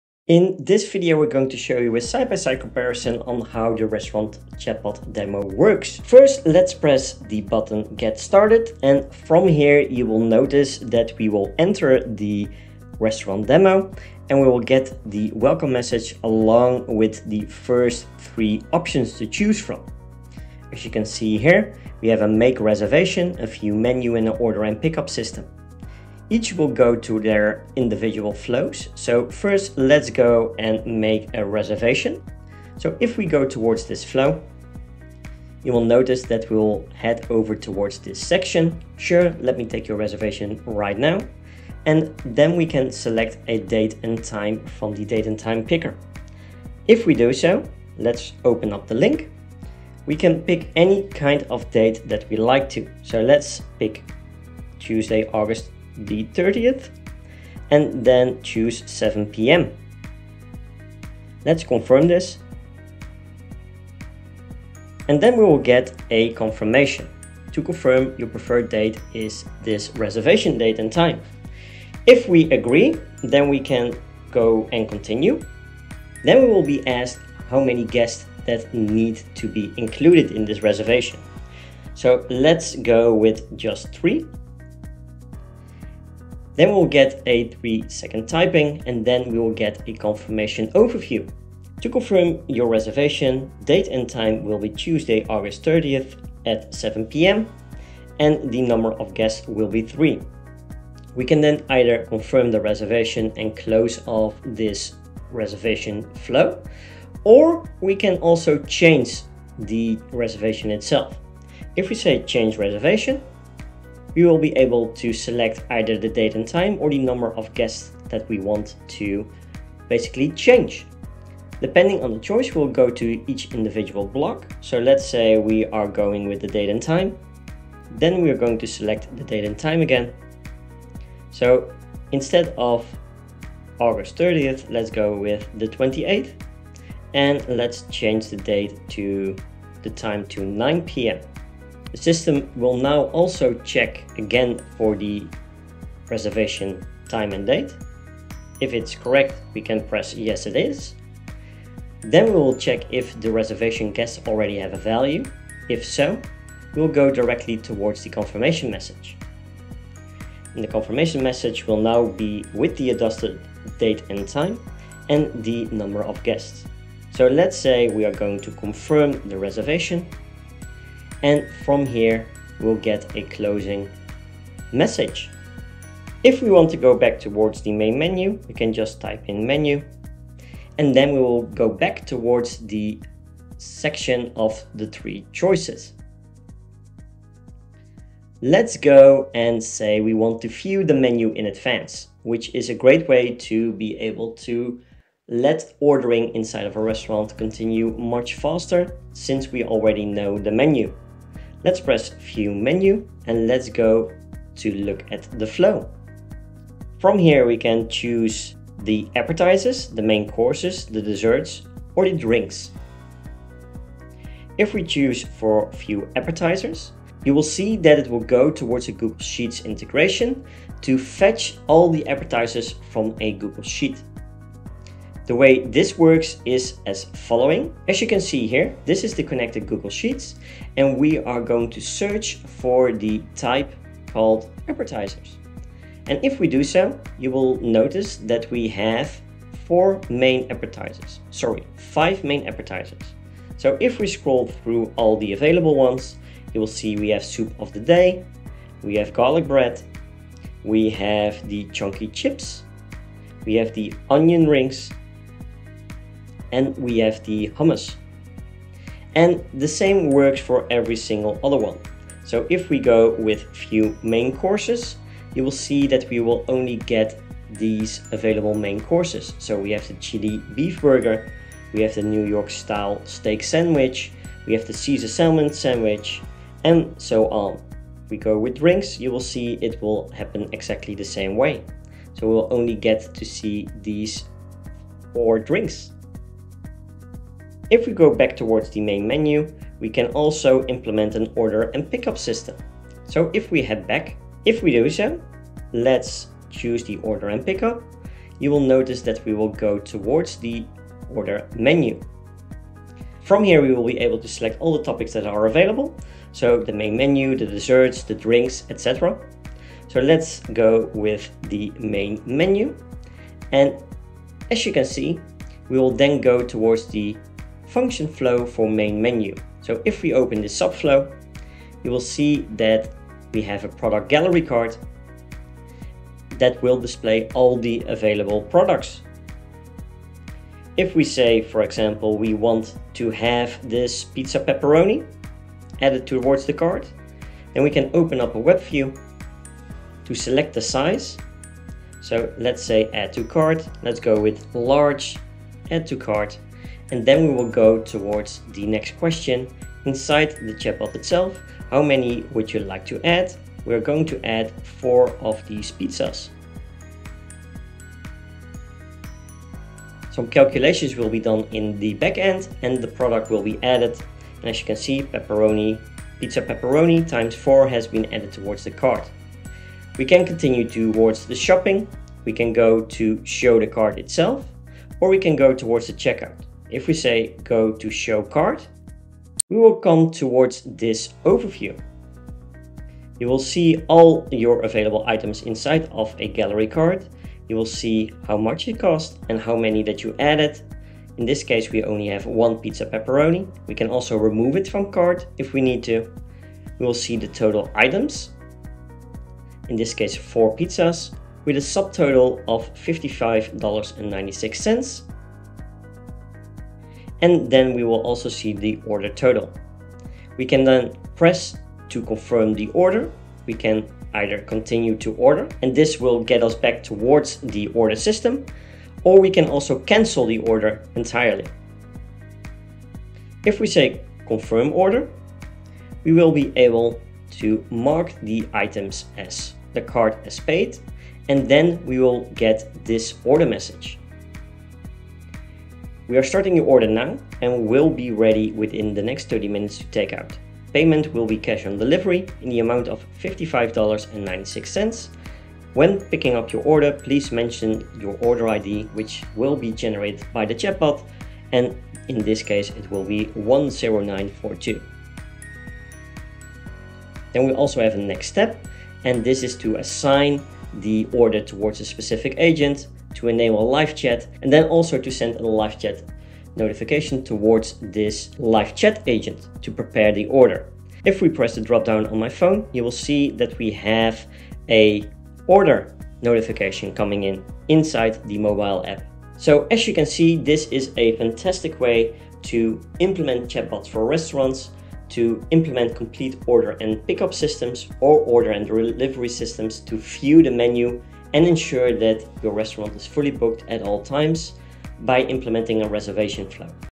UChat restaurant demo overview
A complete overview of how easy you can let a chatbot help your restaurant with a no-code solution